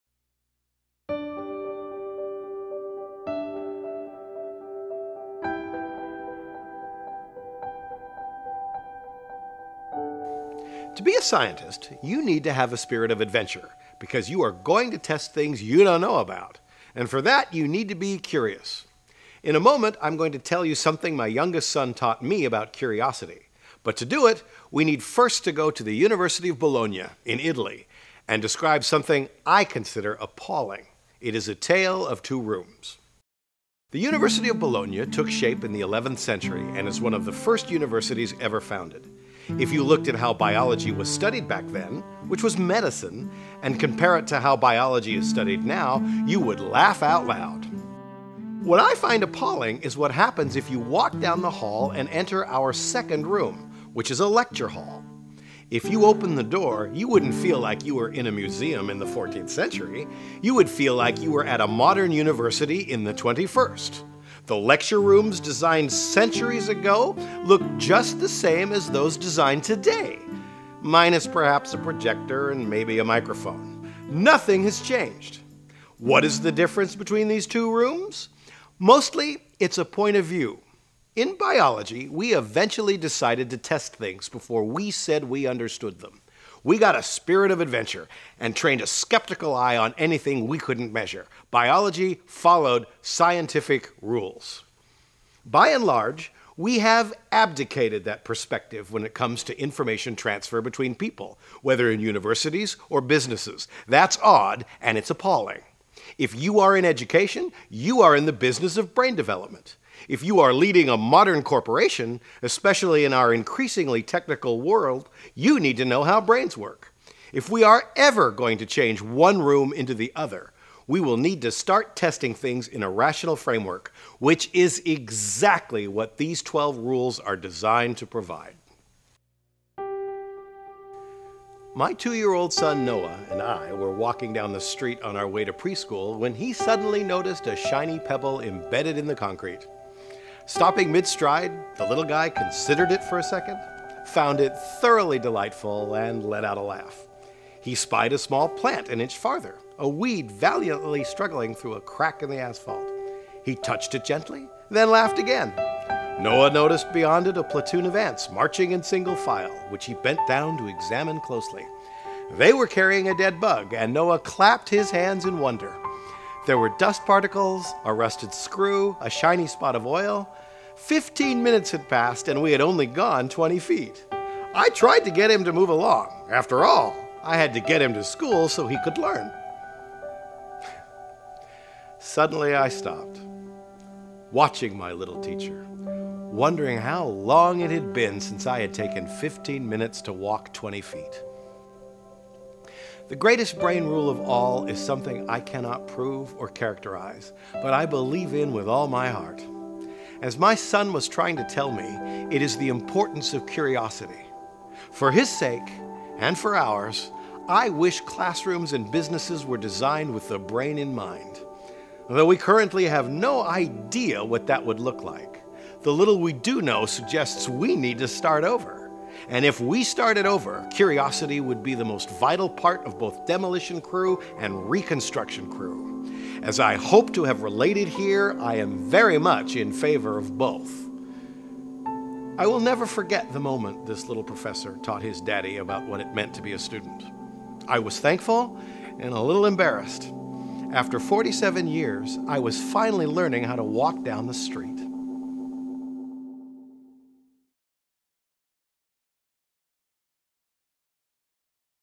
Brain Rule #12: Exploration
John Medina is the author of the New York Times bestseller, "Brain Rules." We are powerful and natural explorers.